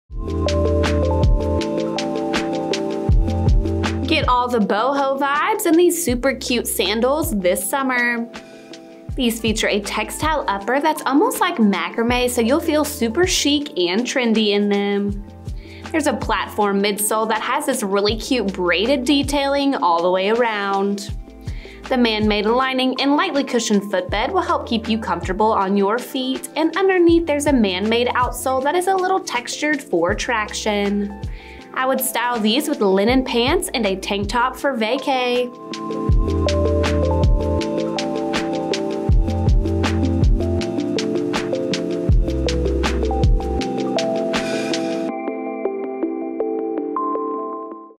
Matisse Kalani SKU: 9669645
Equip your feet with exceptional comfort in true style wearing the Matisse&reg; Kalani Flat Sandals.
Textile upper.
Man-made lining and insole.
Slip-on construction.
Rounded open toe.
Man-made outsole.
Imported.